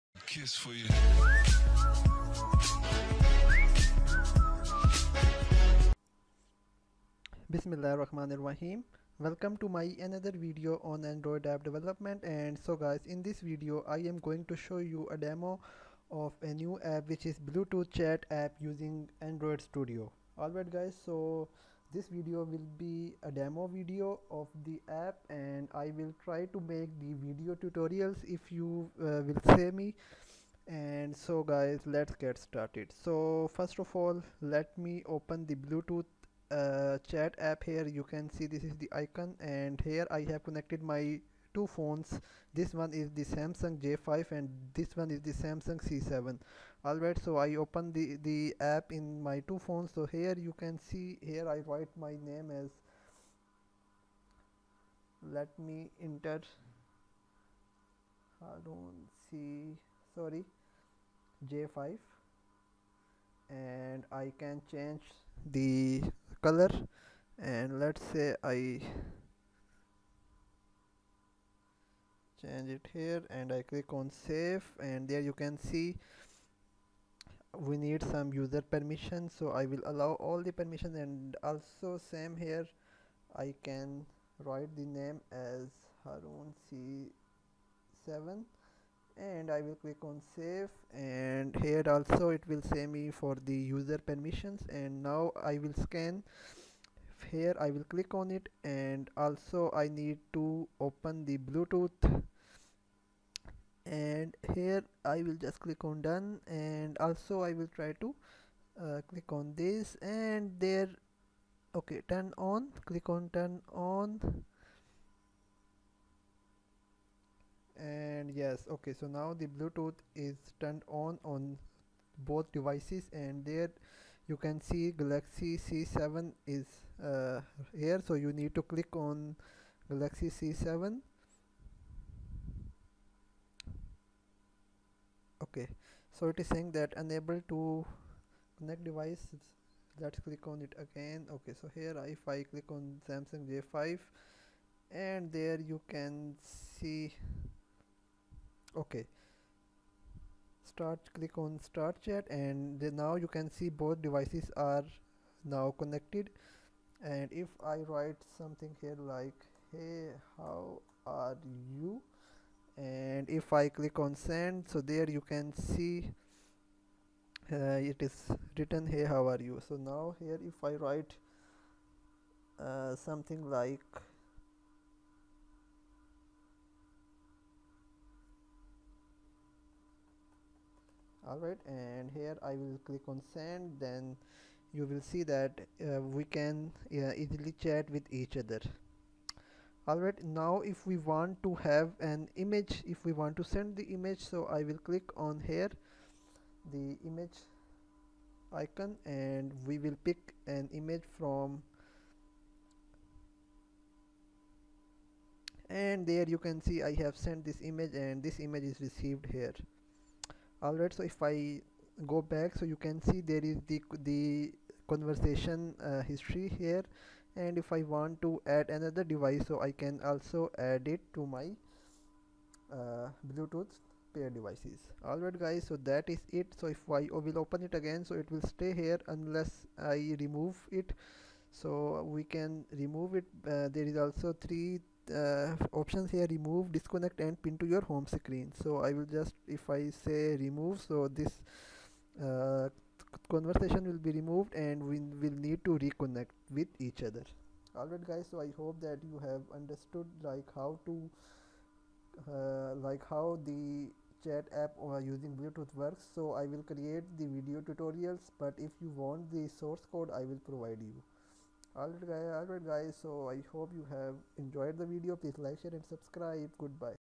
Bluetooth Chat App using Android Studio DEMO
In this video, I am going to give and explain a demo of an app which is Bluetooth Chat app using android studio.

So with this app, you can chat with your friends using bluetooth. Both devices will use bluetooth. They will first connect with each other then they can send messages and pictures to each other using bluetooth in android studio.